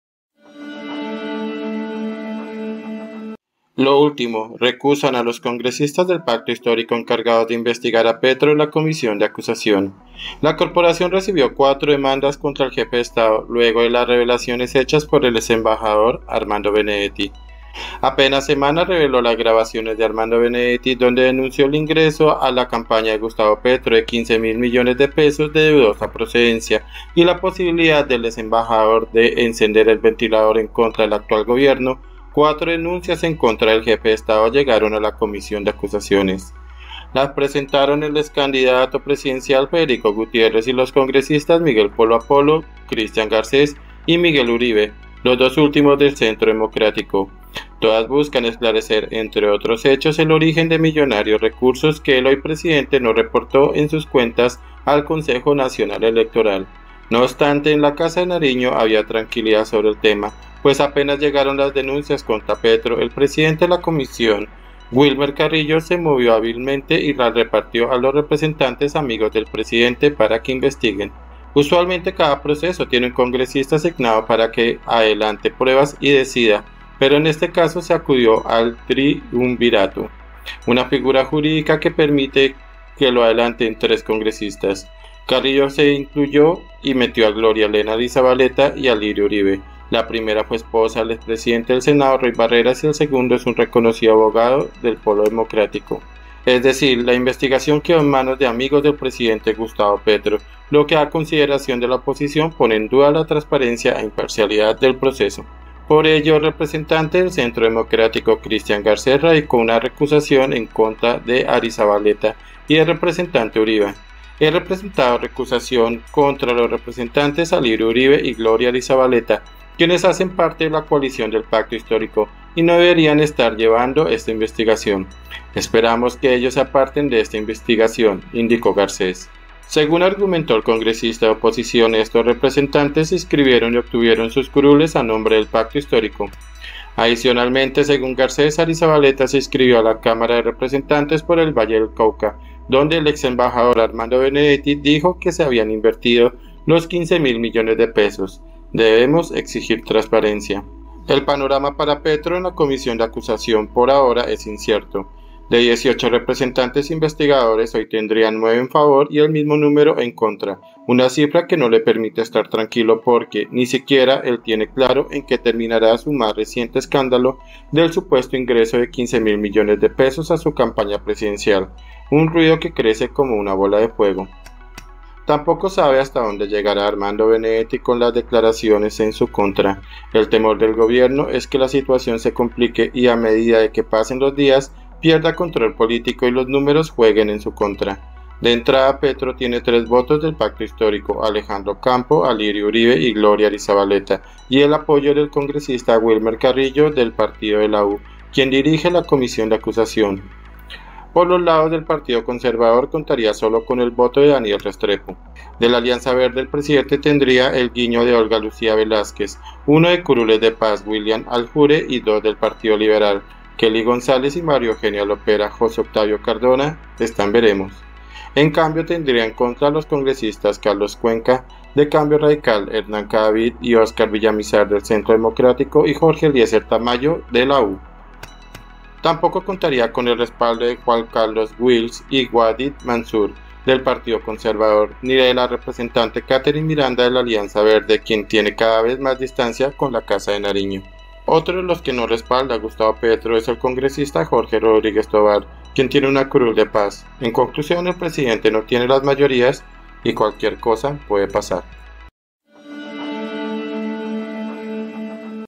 ÚLTIMA HORA 📌 RECUSAN A CONGRESISTAS DEL PACTO HISTÓRICO
“He presentado recusación contra los representantes Alirio Uribe y Gloria Arizabaleta, quienes hacen parte de la coalición del Pacto Histórico y no deberían estar llevando esta investigación. Esperamos que ellos se aparten de esta investigación”, indicó Garcés.